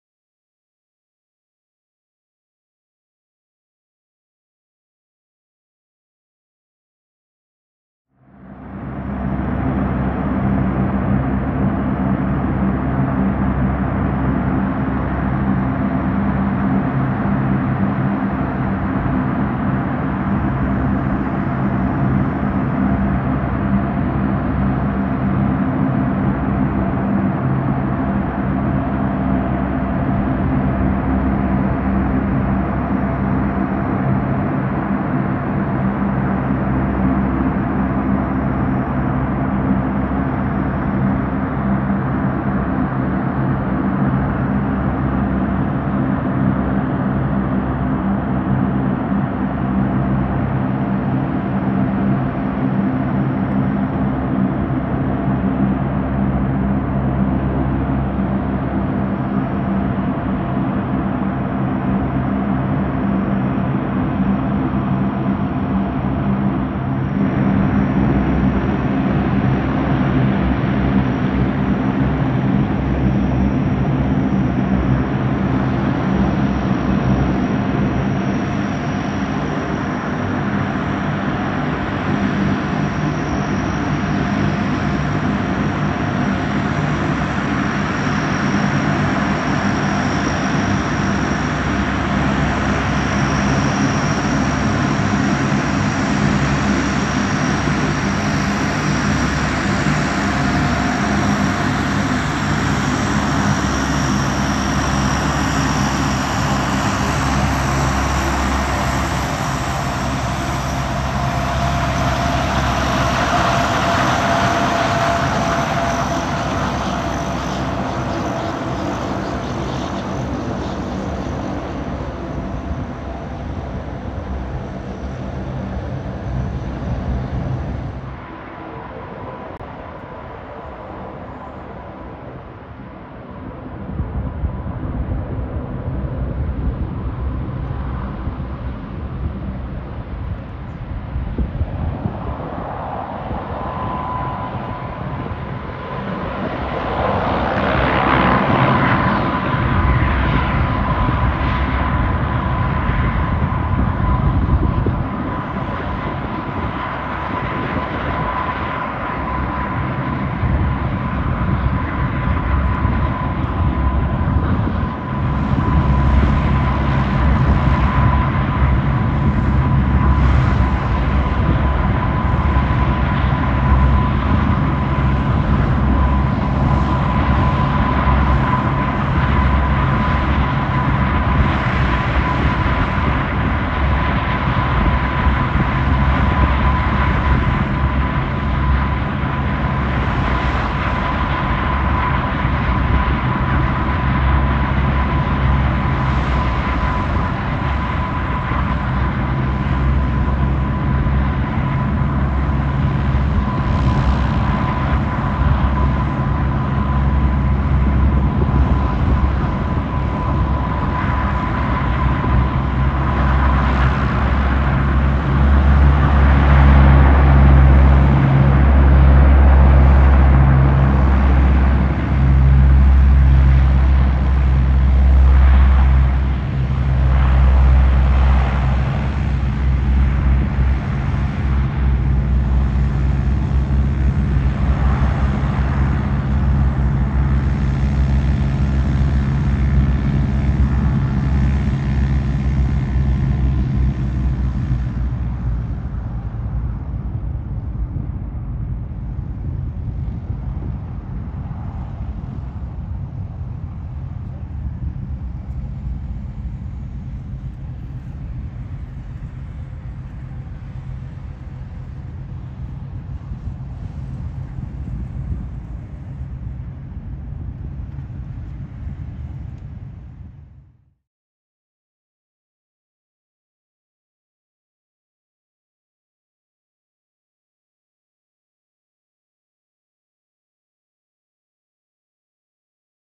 VQ-1 Navy EP3-E Aries II Taxi and Departure | Pease ANGB (PSM)
A Navy EP3-E Aries II from Fleet Air Reconnaissance Squadron VQ-1 at NAS Whidbey Island, taxis and departs from Pease ANGB (Portsmouth International Airport) in Portsmouth, NH.